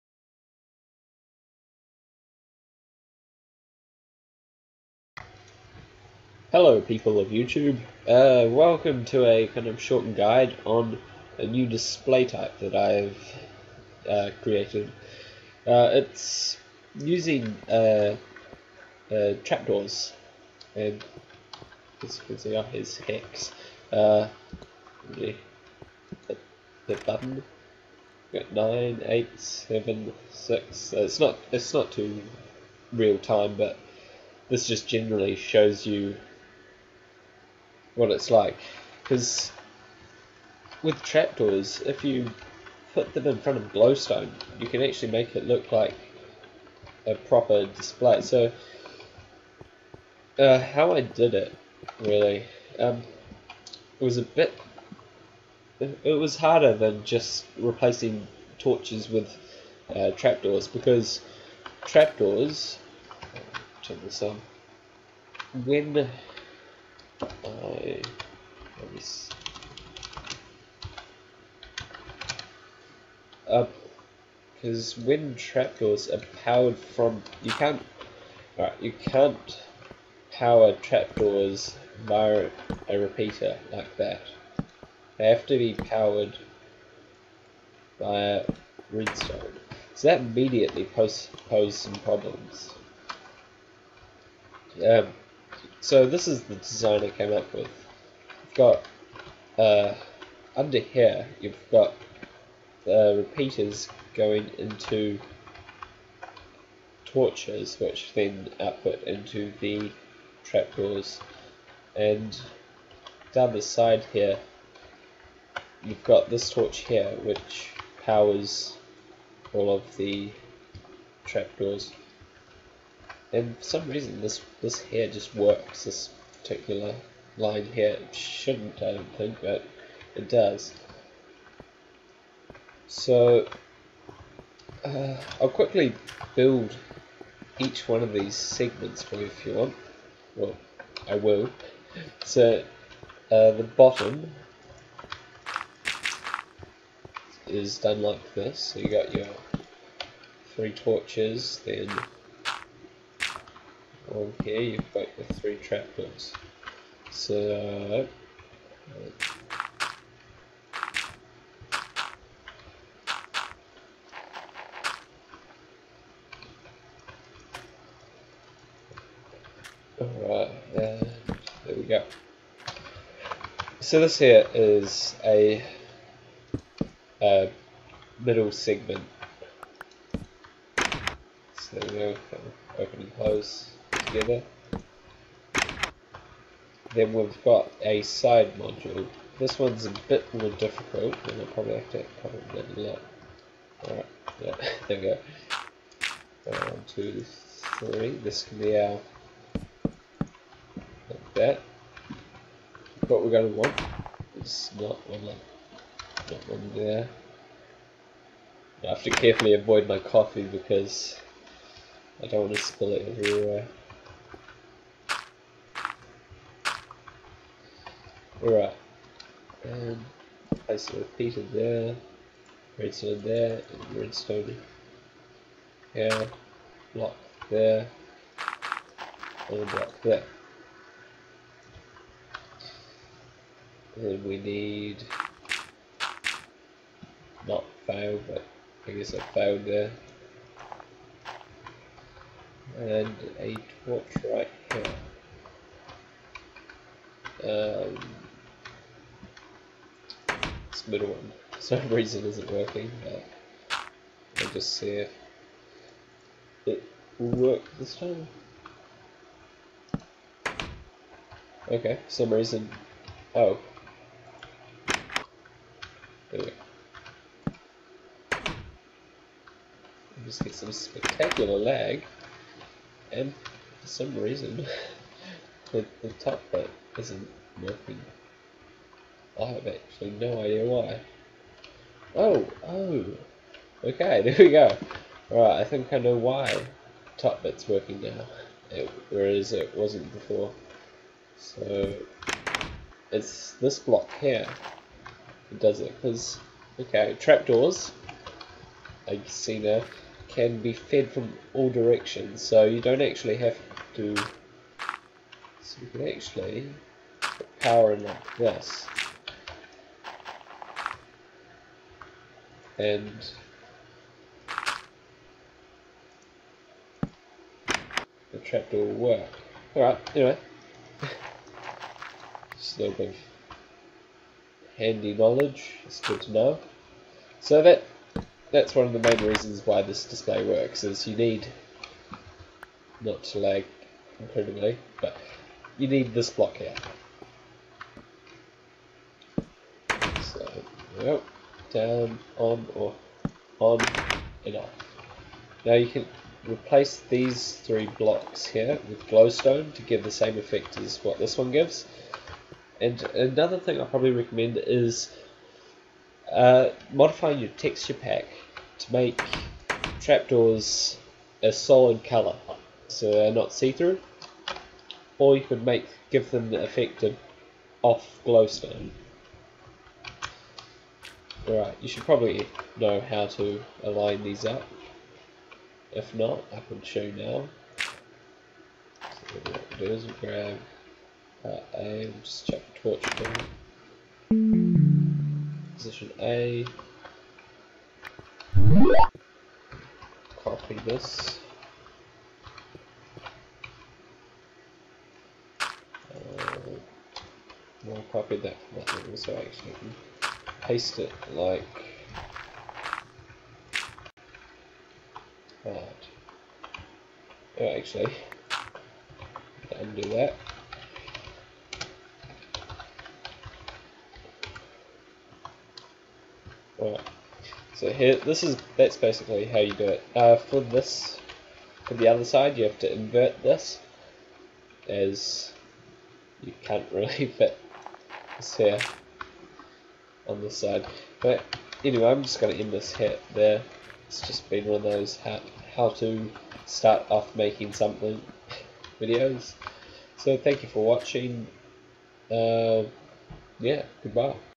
Trap Door 7 segment display [showoff/guide]
Well i finally decided to get around to making a 7 segment display out of trap doors, so here is a showoff video and at the end, a guide on how to make the bars. (also, me finding somthing out about trap doors)